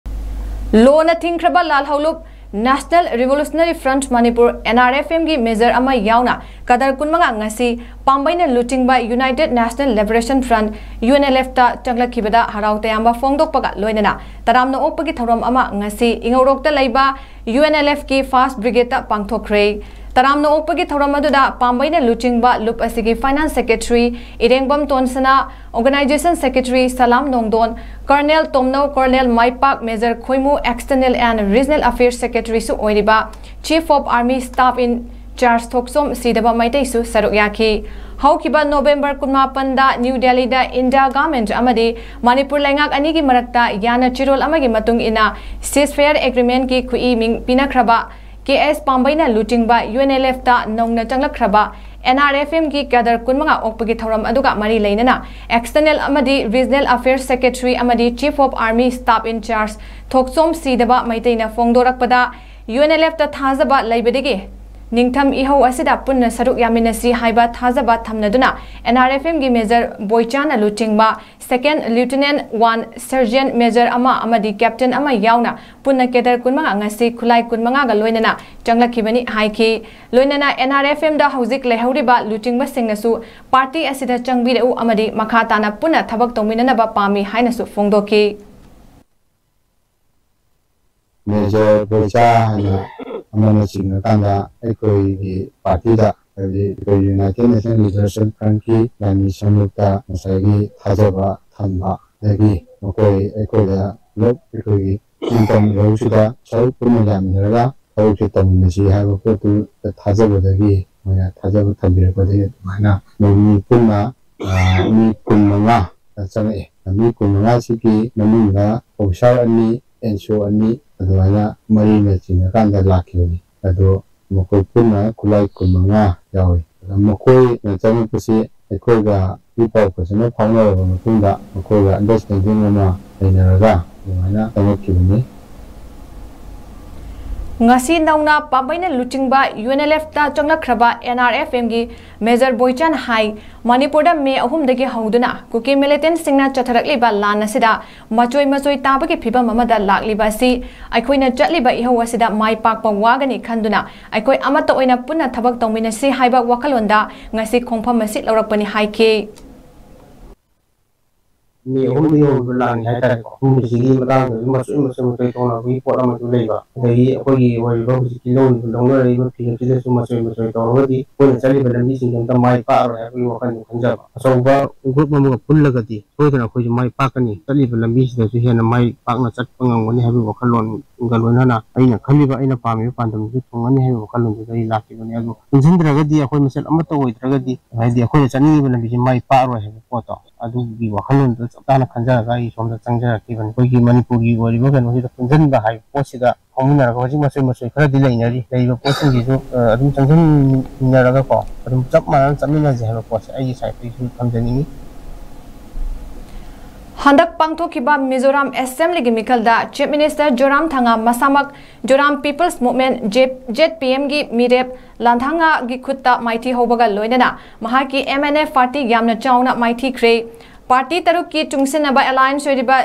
📺𝐍𝐑𝐅𝐌 𝐆𝐈 𝐌𝐀𝐉𝐎𝐑 𝟏 𝐘𝐀𝐎𝐍𝐀 𝐂𝐀𝐃𝐑𝐄 𝟐𝟓 𝐏𝐀𝐌𝐁𝐄𝐈 𝐍𝐀 𝐋𝐔𝐂𝐇𝐈𝐍𝐆𝐁𝐀 𝐔𝐍𝐋𝐅 𝐓𝐀 𝐘𝐀𝐎𝐊𝐇𝐑𝐄 📺
📺𝐍𝐀𝐓𝐈𝐎𝐍𝐀𝐋 𝐑𝐄𝐕𝐎𝐋𝐔𝐓𝐈𝐎𝐍𝐀𝐑𝐘 𝐅𝐑𝐎𝐍𝐓 𝐌𝐀𝐍𝐈𝐏𝐔𝐑 (𝐍𝐑𝐅𝐌) 𝐆𝐈 𝐌𝐀𝐉𝐎𝐑 𝟏 𝐘𝐀𝐎𝐍𝐀 𝐂𝐀𝐃𝐑𝐄 𝟐𝟓 𝐏𝐀𝐌𝐁𝐄𝐈 𝐍𝐀 𝐋𝐔𝐂𝐇𝐈𝐍𝐆𝐁𝐀 𝐔𝐍𝐈𝐓𝐄𝐃 𝐍𝐀𝐓𝐈𝐎𝐍𝐀𝐋 𝐋𝐈𝐁𝐄𝐑𝐀𝐓𝐈𝐎𝐍 𝐅𝐑𝐎𝐍𝐓 (𝐔𝐍𝐋𝐅) 𝐓𝐀 𝐘𝐀𝐎𝐊𝐇𝐑𝐄; 𝐏𝐀𝐑𝐓𝐘 𝐆𝐈 𝐋𝐔𝐂𝐇𝐈𝐍𝐆𝐁𝐀𝐒𝐈𝐍𝐆𝐍𝐀 𝐌𝐀𝐊𝐇𝐎𝐈𝐁𝐔 𝐓𝐀𝐑𝐀𝐌𝐍𝐀 𝐎𝐊𝐊𝐇𝐑𝐄 📺